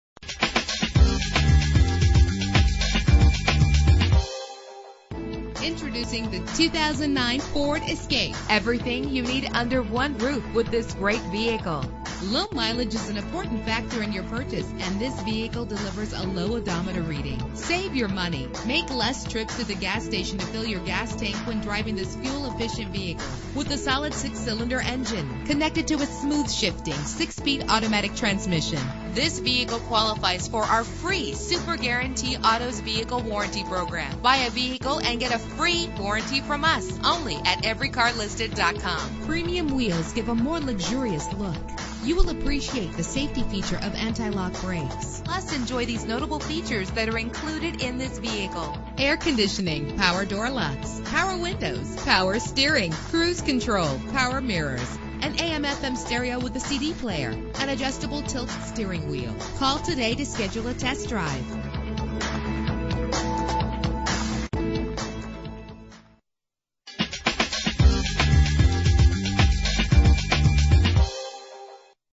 2009 Ford Escape - Jefferson City MO
Year: 2009
Make: Ford
Model: Escape
Trim: XLT
Engine: 3.0 liter 6 cylinder 24 valve
Transmission: 6-Speed Automatic
Color: Silver
Mileage: 18779
Address:
2033 Christy Dr